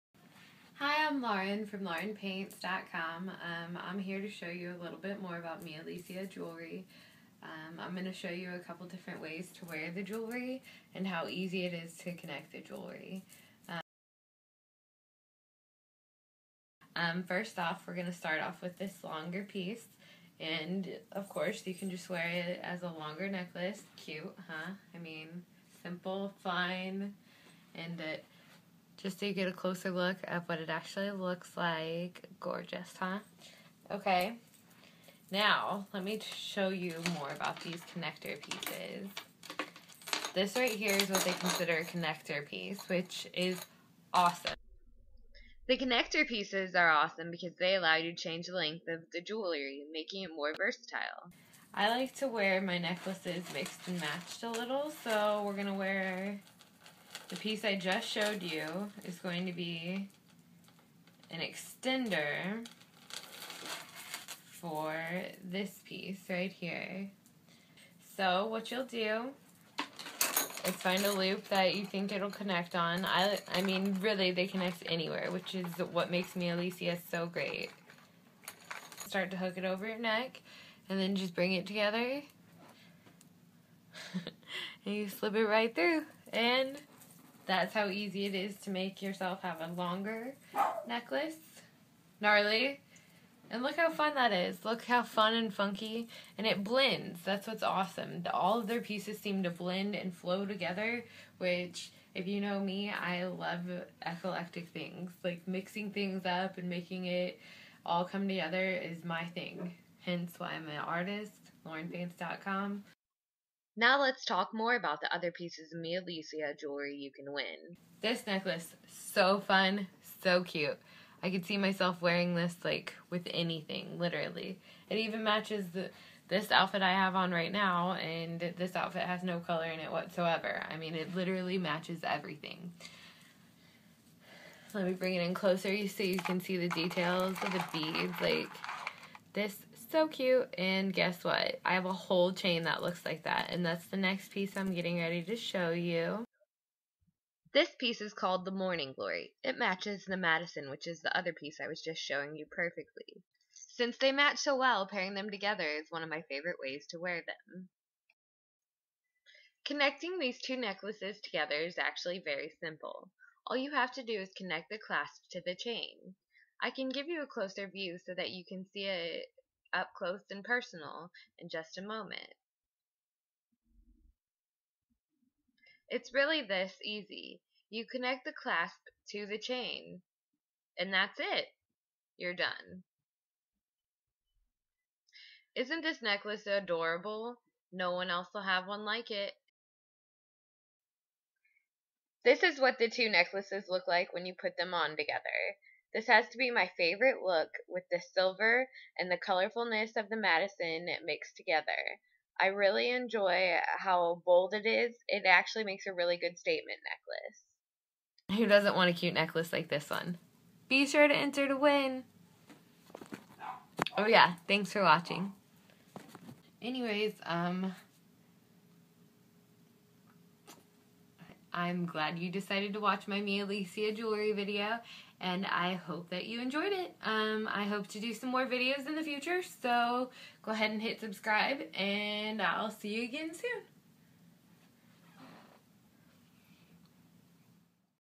Mialisia Jewelry - How to Style - Lauren Paints | a beautiful life
My first vlog: Have you heard of Mialisia Jewelry?

In this video I share how I style my Mialisia Jewelry pieces, helping to explain how the simple clasp and connector system work... Mialisia is the best accessories line and lucky for you, you have the chance to win this set of necklaces! Head on over to LaurenPaints.com for more information!